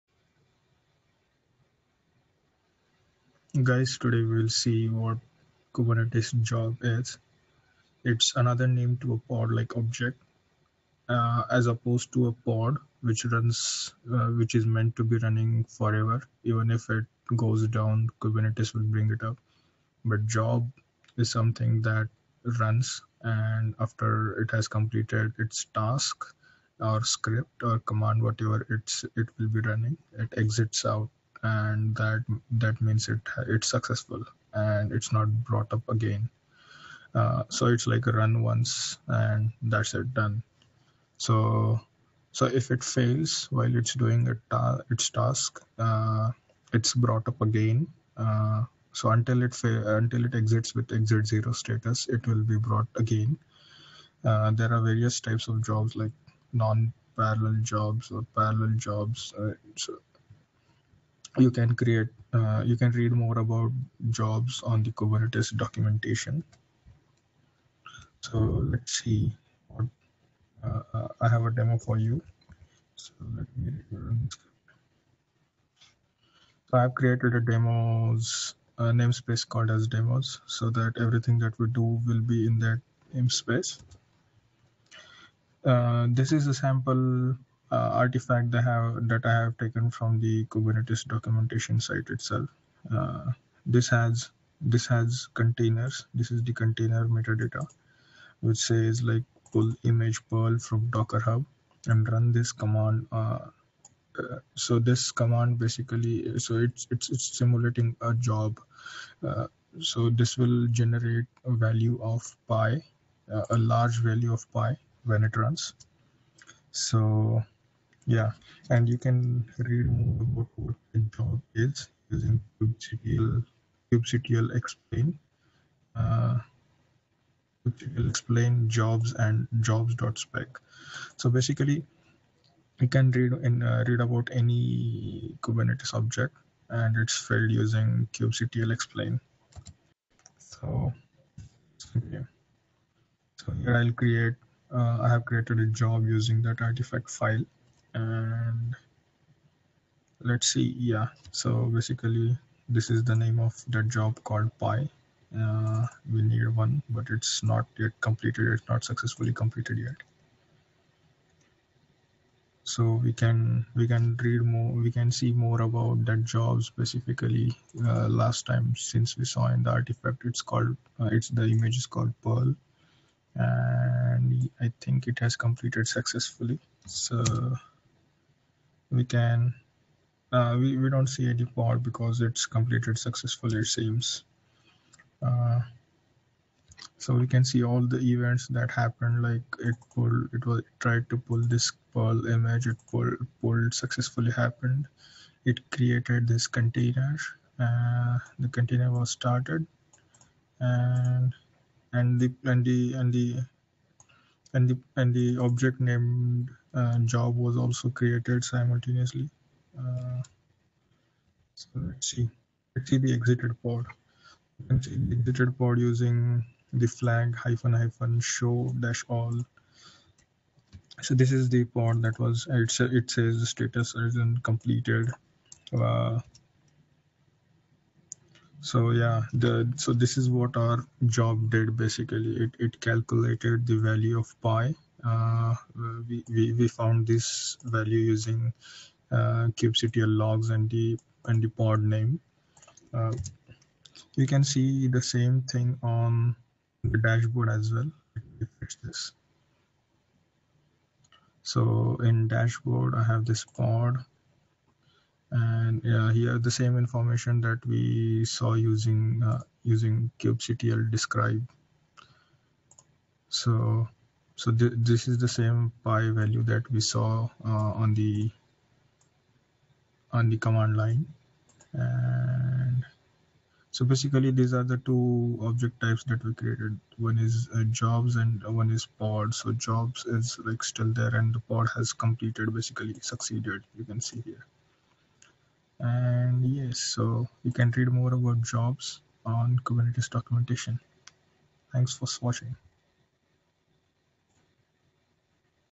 Kubernetes Job
In this video you can see what is Kubernetes Job is? And a demo of it.

and goto folder `jobs`